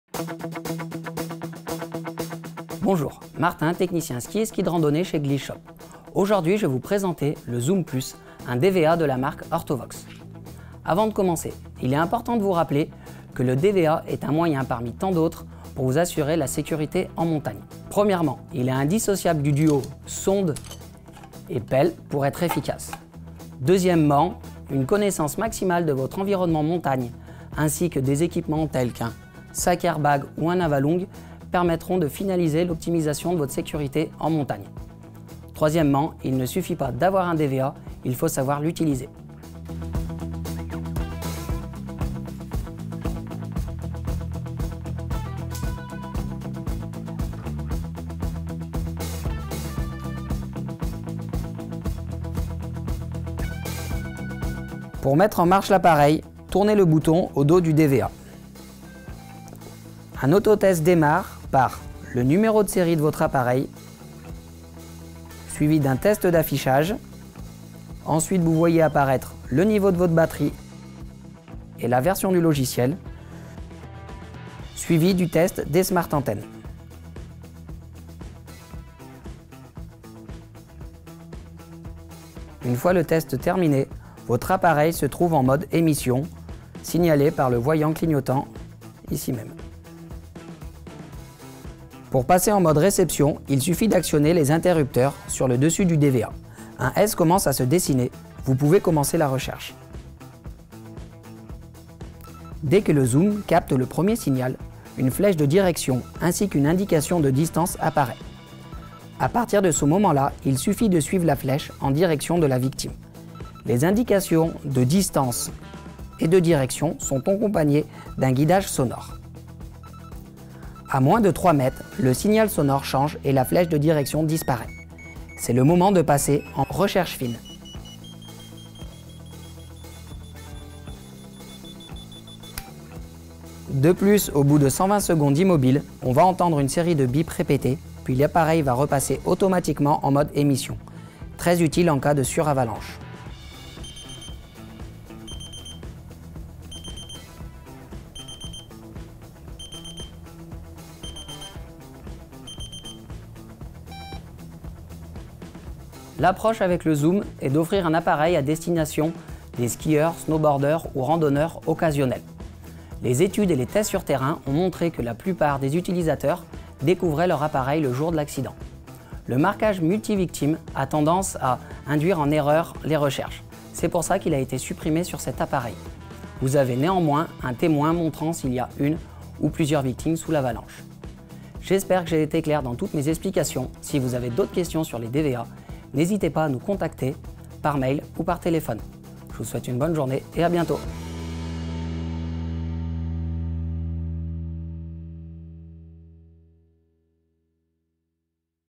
Présentation DVA Ortovox Zoom +
Martin nous présente les différentes fonctions du DVA Zoom + de chez Ortovox. Ce modèle est simple d'utilisation pour une recherche de victimes en avalanche intuitive et rapide.